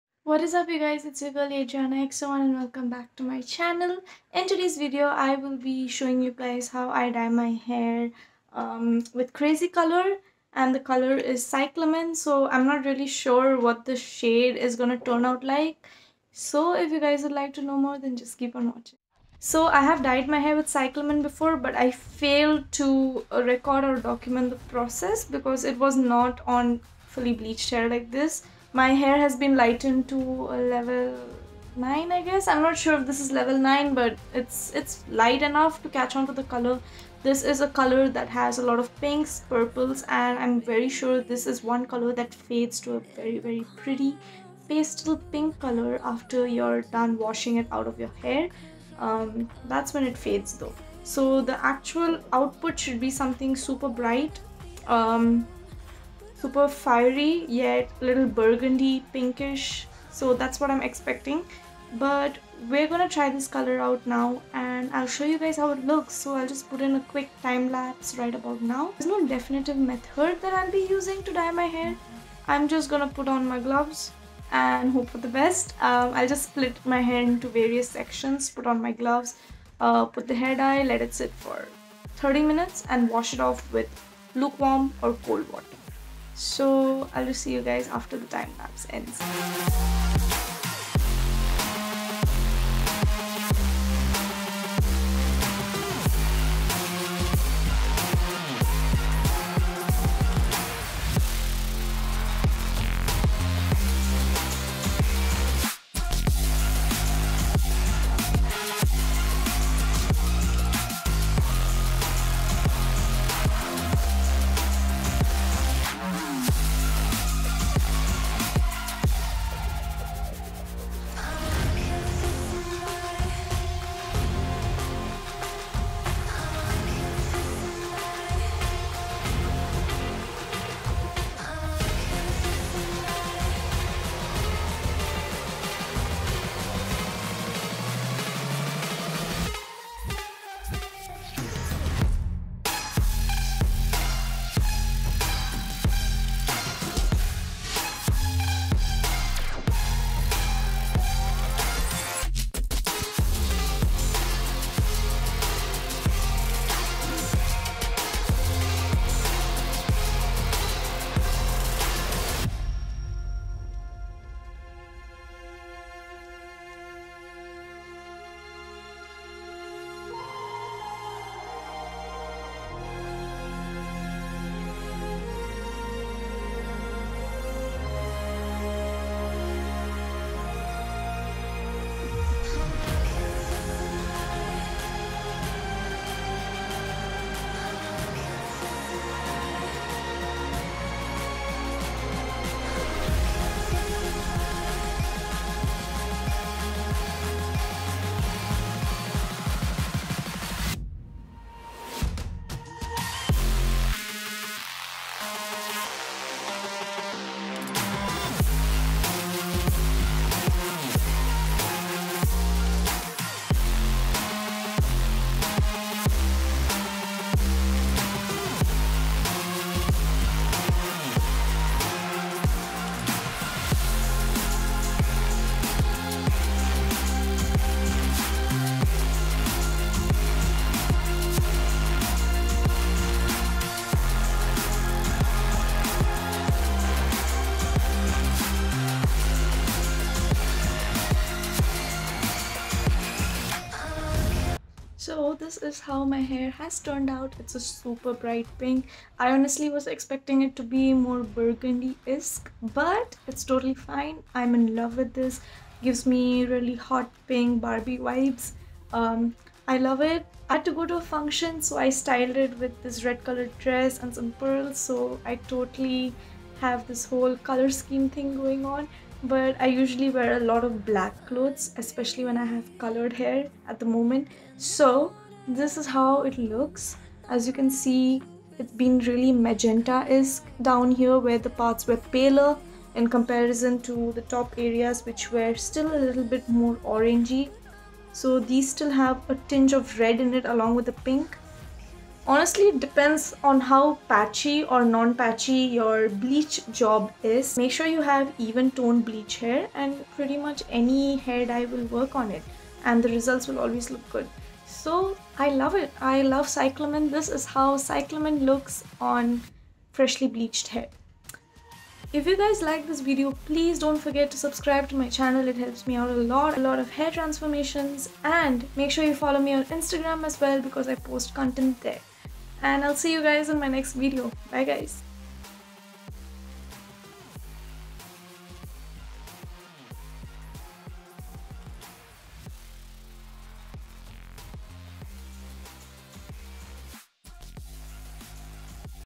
How I Dyed my hair PINK with Crazy Color Cyclamen
So, I finally tried my hands on the crazy color Cyclamen dye for the first time since i wanted to do a colour i've never tried before. And this is how it turned out.

My next upload is going to be a tutorial on how i lightened my hair with minimal damage at home.